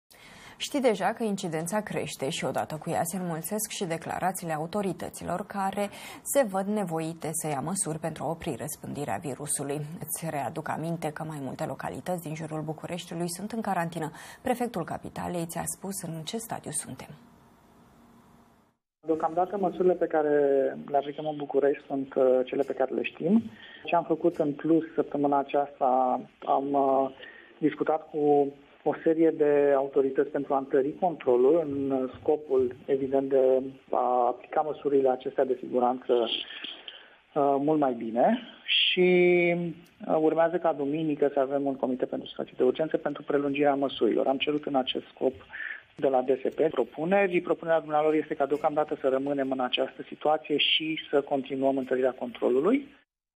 Bucureștiul, din nou în carantină? Declarația Prefectului
Bucureştiul are o incidenţă de 4,6 la mia de locuitori şi 1031 de cazuri noi înregistrate în ultimele 24 de ore.

”Deocamdata măsurile pe care le aplicăm în Bucureşti sunt cele pe care le ştim. Ce am facut în plus săptămâna aceasta a fost să discut cu o serie de autorităţi pentru a întări controlul cu scopul ca aceste măsuri să fie implementate mai bine”, a spus Alin Stoica la Aleph News.